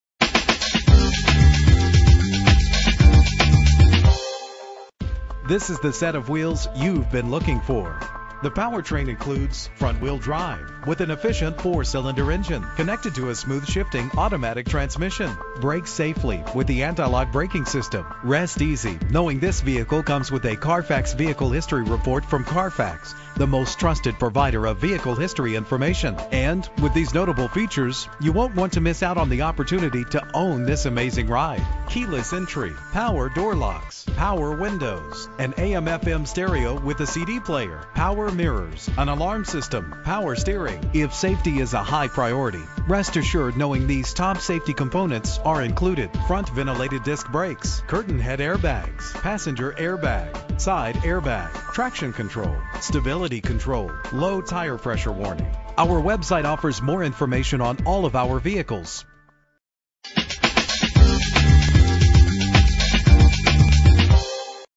2012 Nissan cube - MEDINA OH
Year: 2012
Make: Nissan
Model: cube
Trim: 1.8 S
Engine: 1.8 liter inline 4 cylinder 16 valve
Transmission: Automatic CVT
Color: Pearl White
Mileage: 3
Address:
5180 MONTVILLE DR